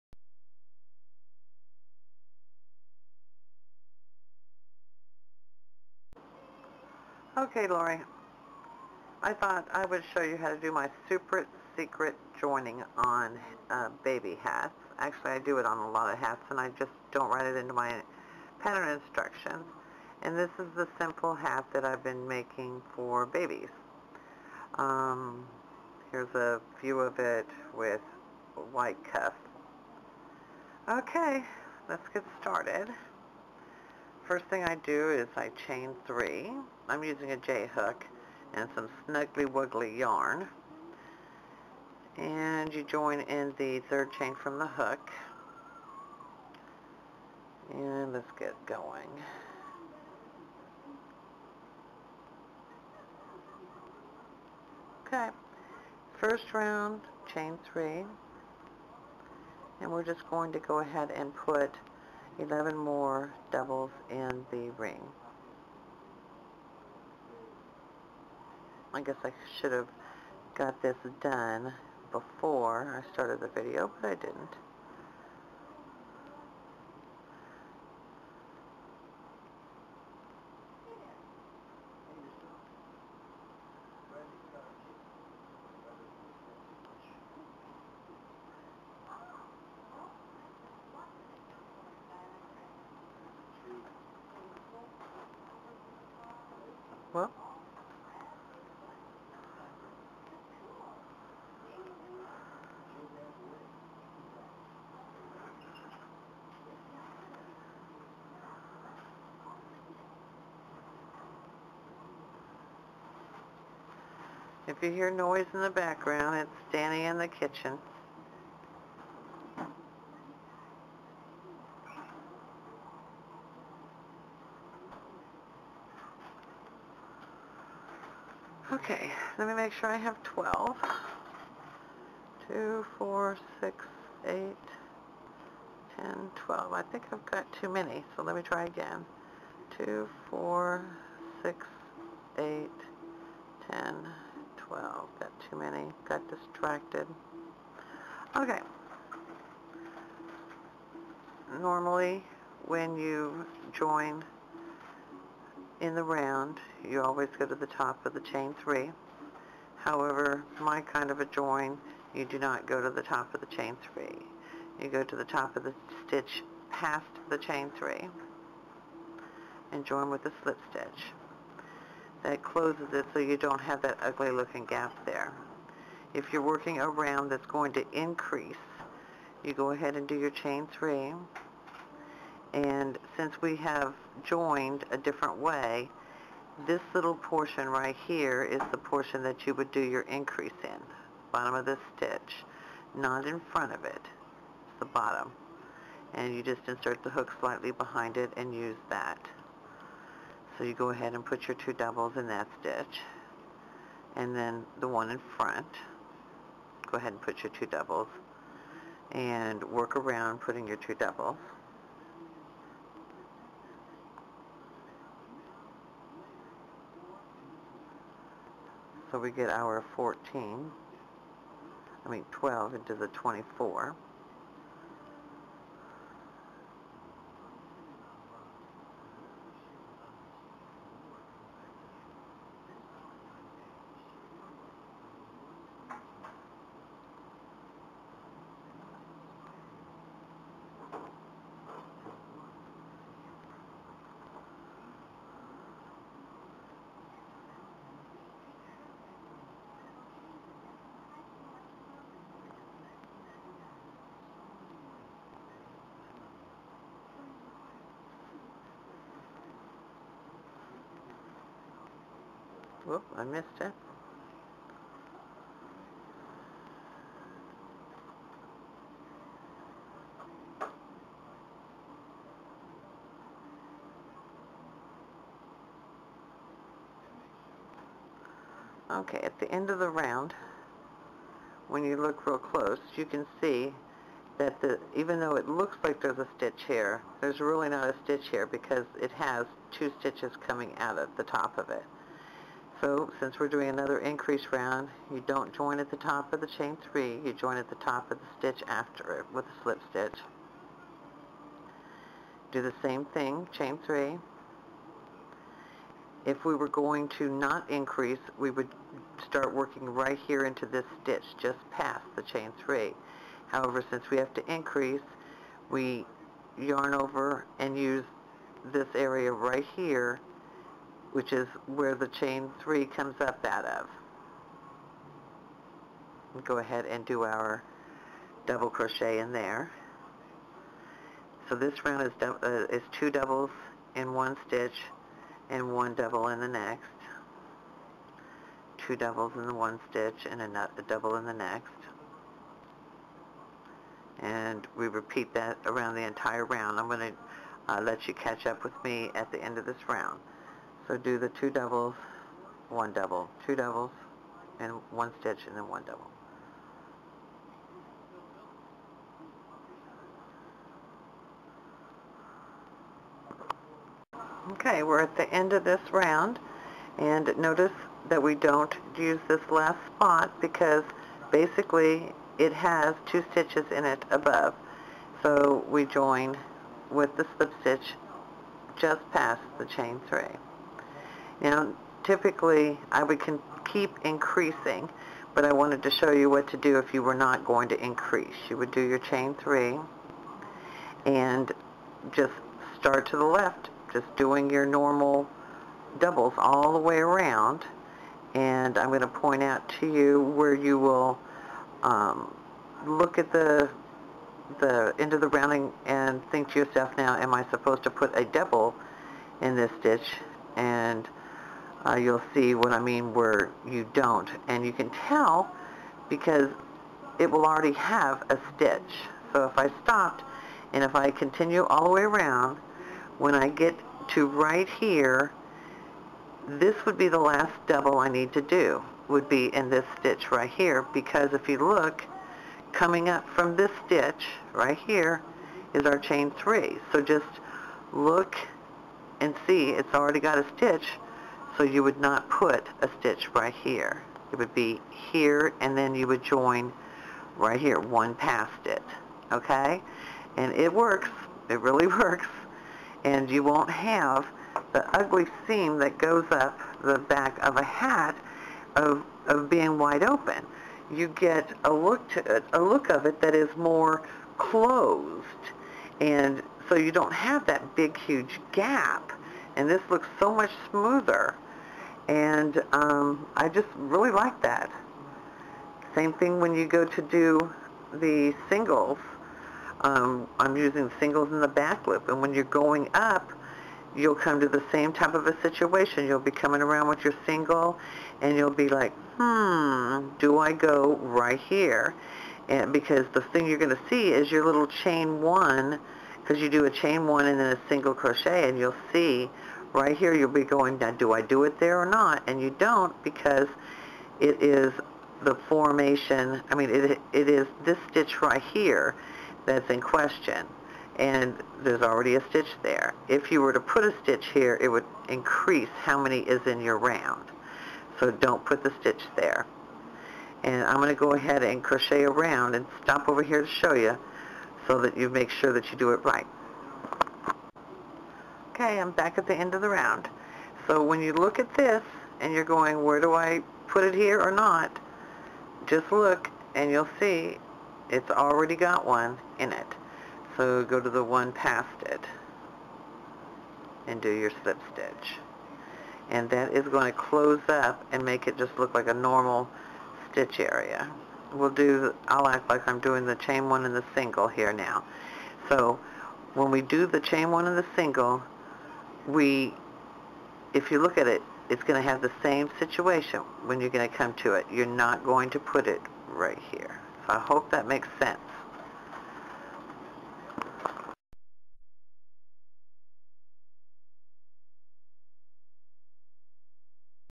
The SmoothFox Join Technique
This is a video with the secret way that I join circular items in the round or join hats in the round.  Not really a secret LOL but it was just a technique that I have used and never told anyone about it.